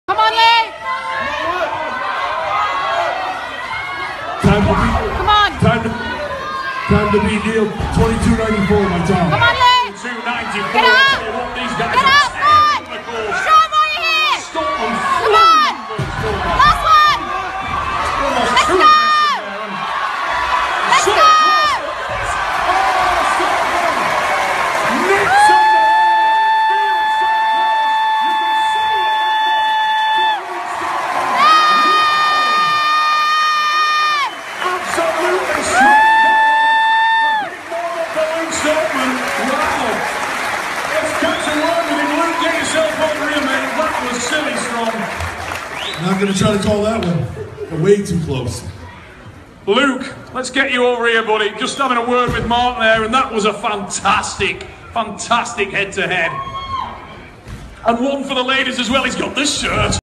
Luke Stoltman Atlas Stone run Britain's Strongest Man 2017
My winning atlas stone run at Britain's strongest man in Doncaster in 2017. Beating Eddie Hall😳🙄